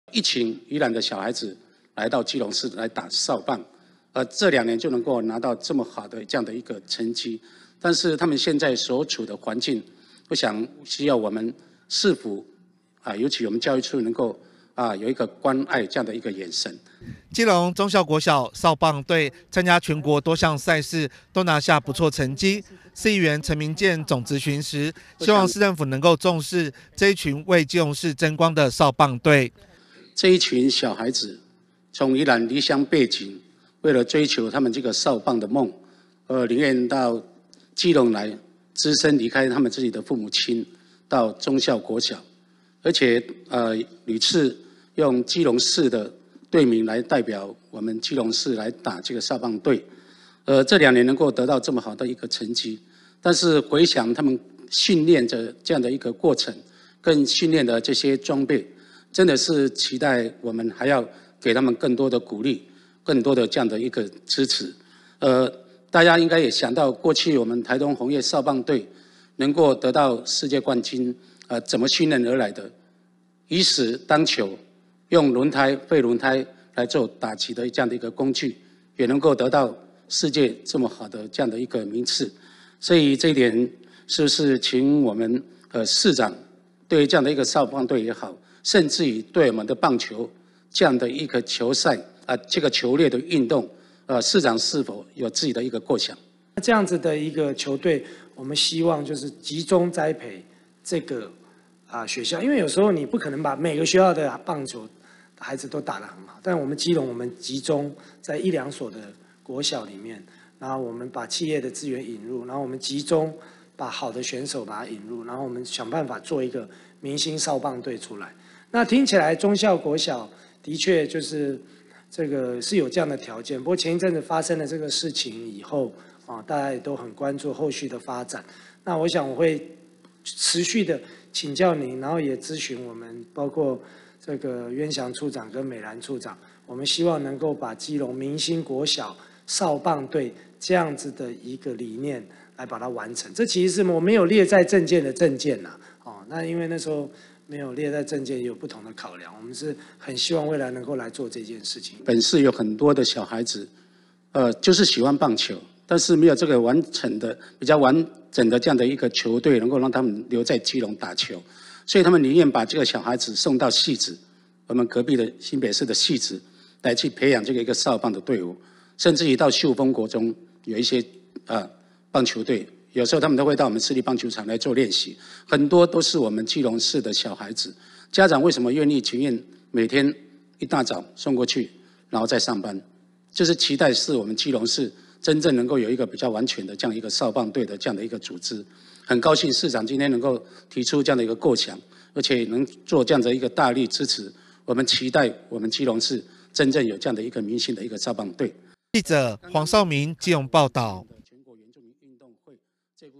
明星少棒隊｜培養明星少棒隊 陳明建盼市府重視棒球運動
(中嘉吉隆新聞 記者黃紹銘/基隆報導) 基隆忠孝國小少棒隊，參加全國賽事都有不錯成績，全校學生僅70人，卻誕生了全基隆唯一能於全國賽與他校抗衡的少棒隊。基隆市議員陳明建替少棒隊請命，希望市長謝國樑能重視少棒隊的成績，好好培養屬於基隆市少棒隊，謝國樑說，雖然沒有納入競選政見，但未來會引進企業來贊助，好好陪培養優秀選手。

基隆忠孝國小少棒隊，參加全國多項賽事，都拿下不錯成績，市議員陳明建總質詢時，希望市政府重視，這一群為基隆市爭光的少棒隊。